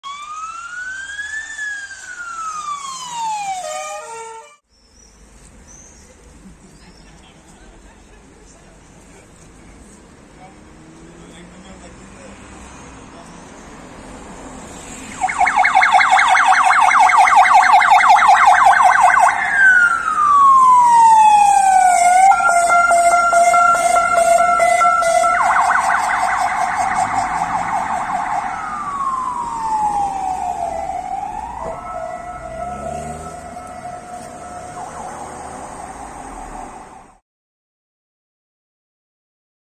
[ALARMOWO] [KREW] [OPOLE] Alarmowy przejazd krwi NA RATUNEK - RAPTIM. Opole, 06.07.2022r.
Alarmowy przejazd transportu krwi na ratunek z Regionalnego Centrum Krwiodawstwa i Krwiolecznictwa w Opolu wykonywany przez firmę RAPTIM ze śląska.